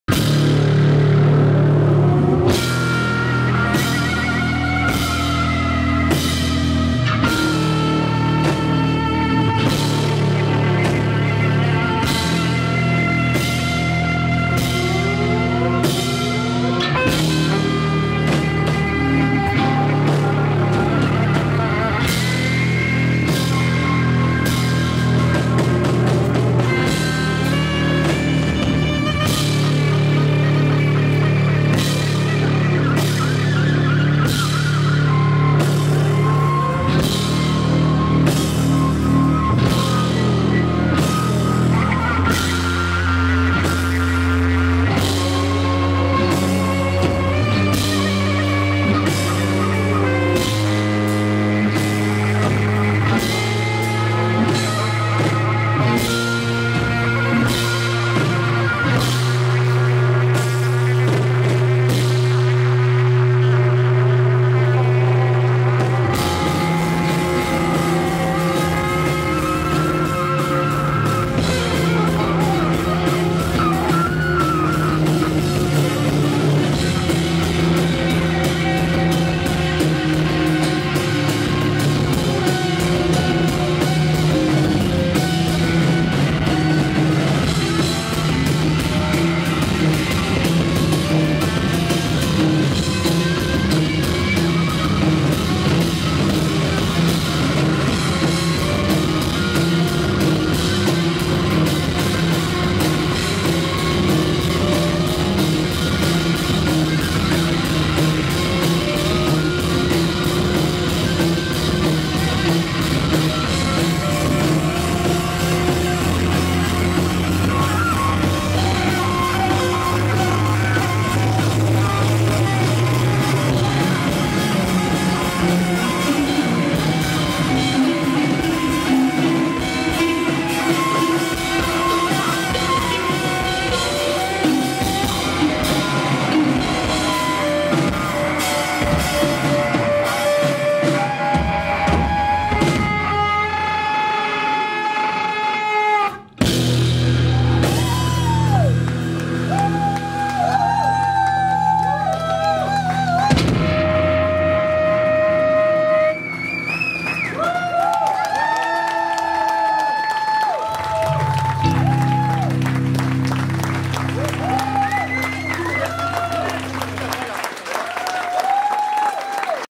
Oiseaux-Tempête feat. Mondkopf live @ Le Kalif (Rouen)
In  O I S E A U X - T E M P Ê T E ([wazo-tɑ̃.pɛːt] - Storm Petrel),Frédéric D. Oberland and Stéphane Pigneul have chosen a name, a call from the sea, of almost totemic quality. First, perhaps, it offers the suggestion of journeys over great distance, the distant hymns of fortune-tellers and gravitydefying acts of the oracles. Yet at the same time, comes the idea of earthly chaos, a willing reference to the telluric frenzy that darkens the sky yet also, paradoxically, brings promise of a radiant tomorrow.